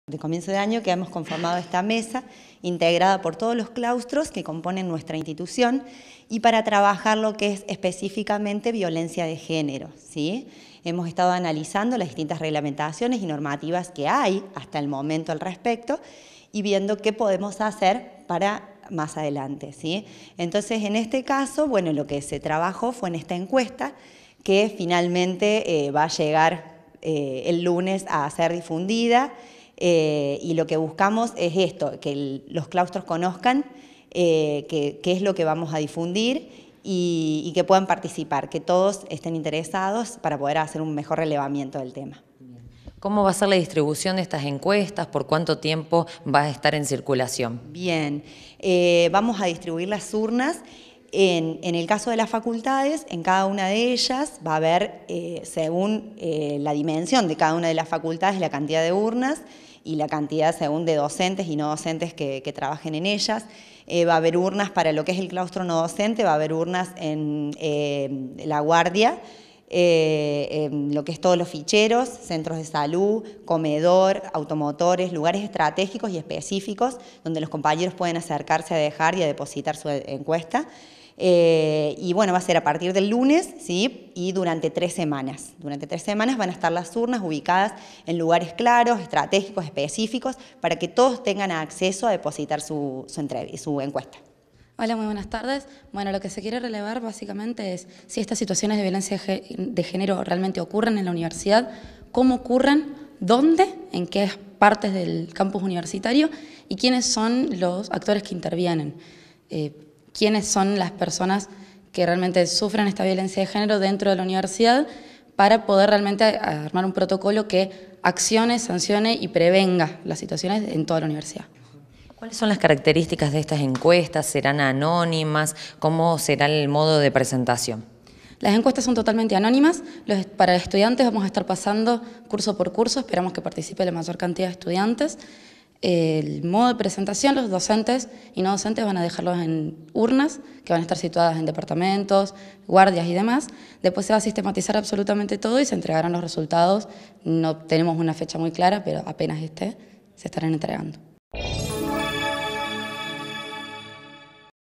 RELEVAMIENTO SOBRE VIOLENCIA DE GÉNERO - ANA SCANDURA Y MICAELA GUMAN
DEL 22/10 AL 09/11 REALIZARÁN ENCUESTAS A LA COMUNIDAD UNIVERSITARIA
Facebook.com/uniriotv
Twitter.com/uniriotv